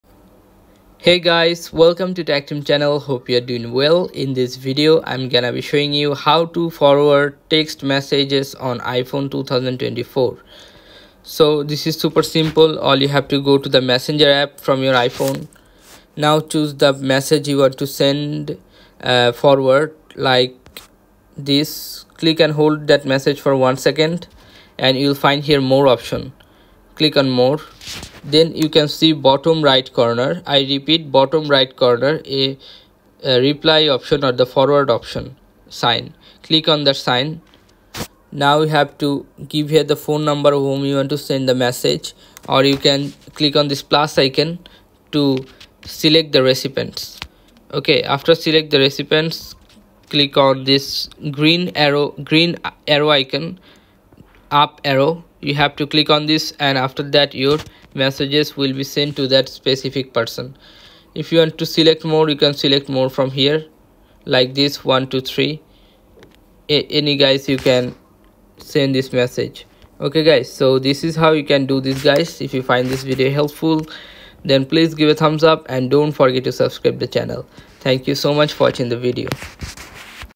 The iPhone 2024 Text Message Forwarding HACK You Need
Unlock the hidden potential of your iPhone with this text message forwarding hack for 2024. Stay ahead of the game with this game-changing trick!